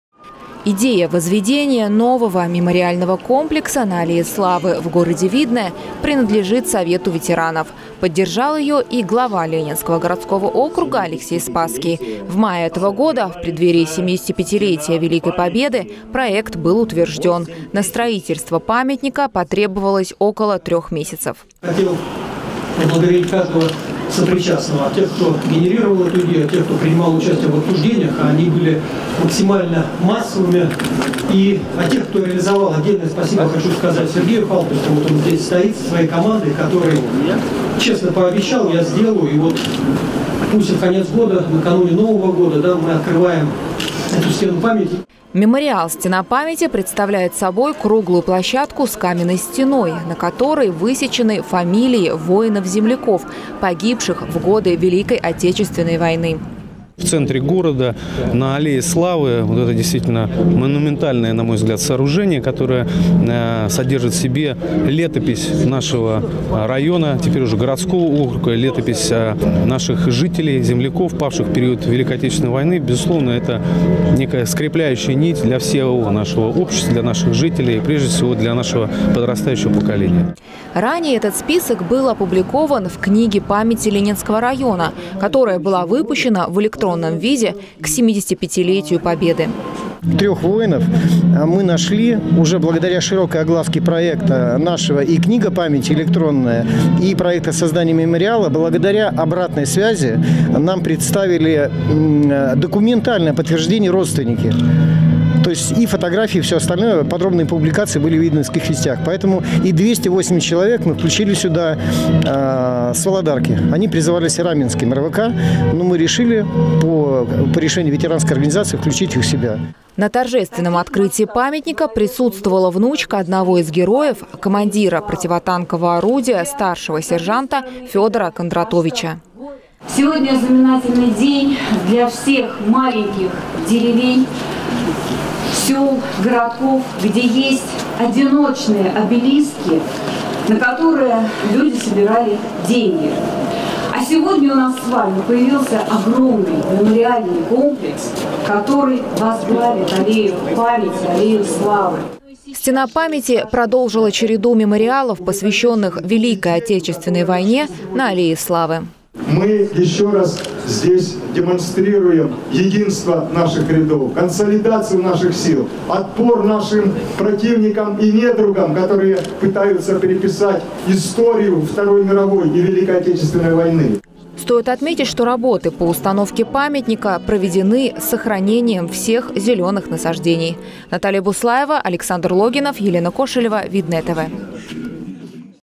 «Стена памяти» - открытие мемориала на Аллее Славы
На аллее славы состоялось торжественное открытие мемориала «Стена памяти», установленного в честь 75-летия Победы и посвященного жителям Ленинского городского округа павшим в годы Великой Отечественной войны.